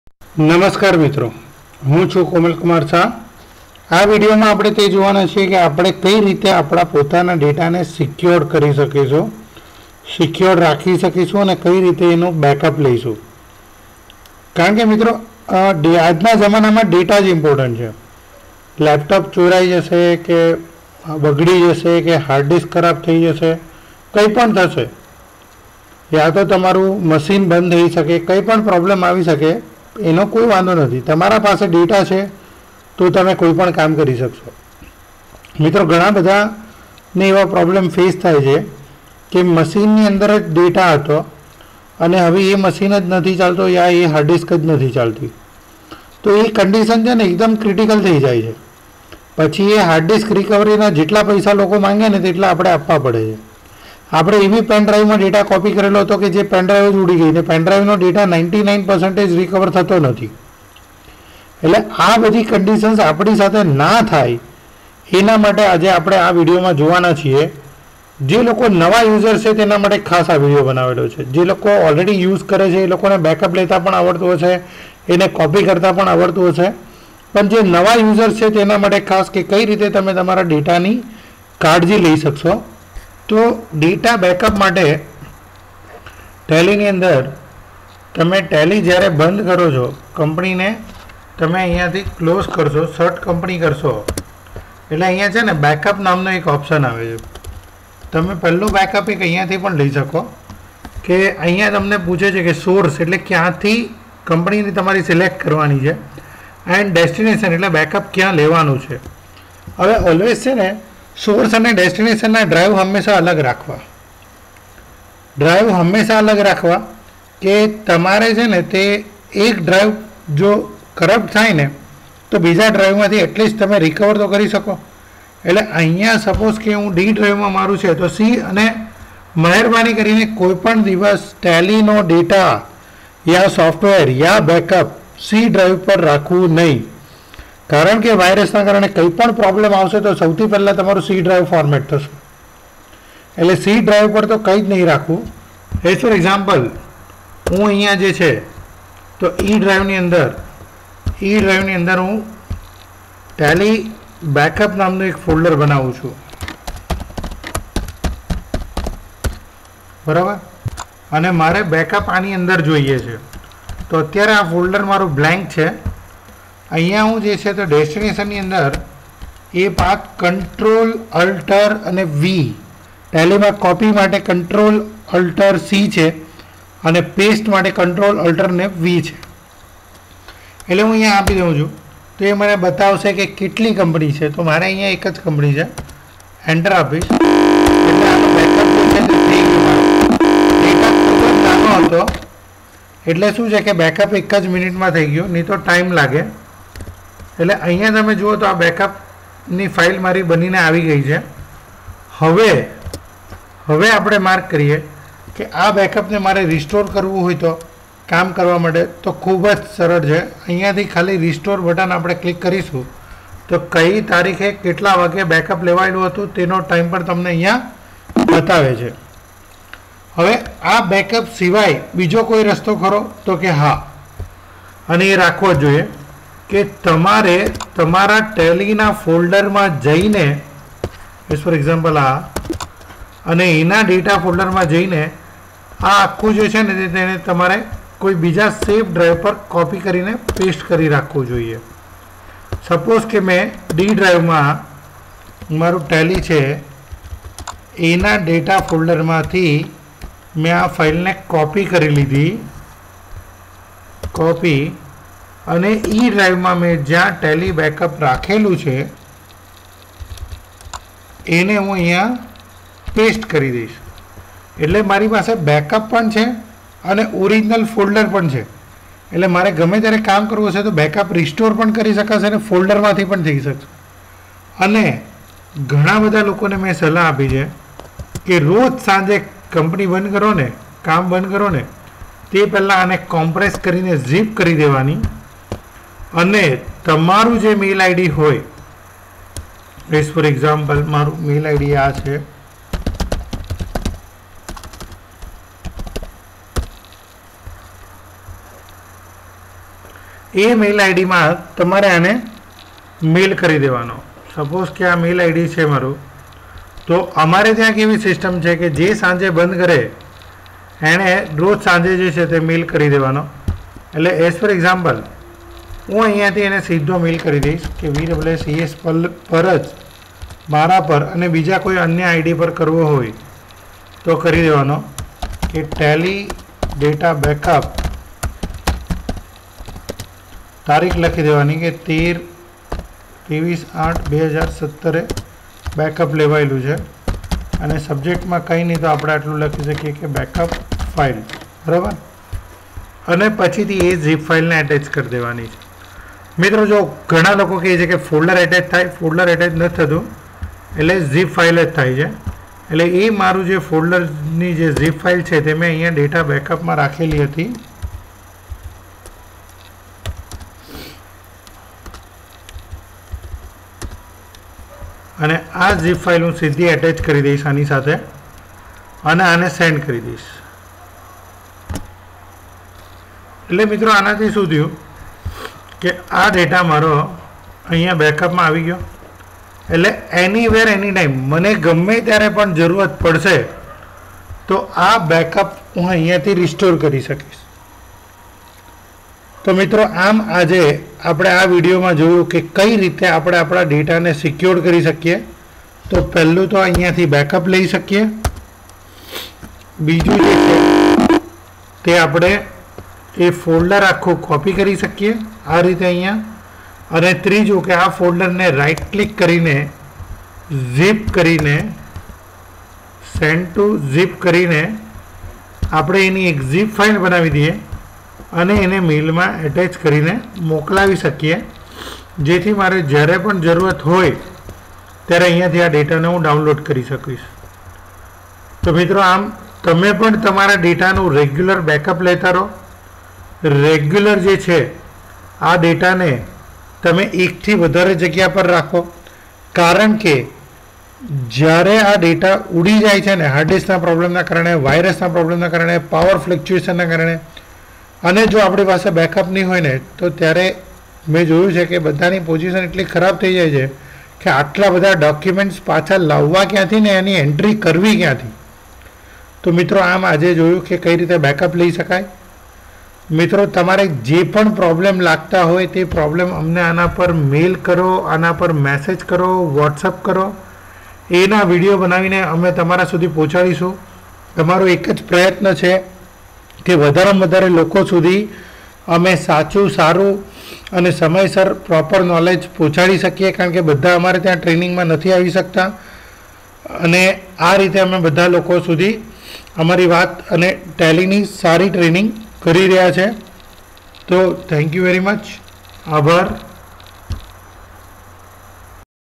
Tally Data Backup
This video explain you about How to take backup from Tally.ERP9 Software and what are the other way  to take backup.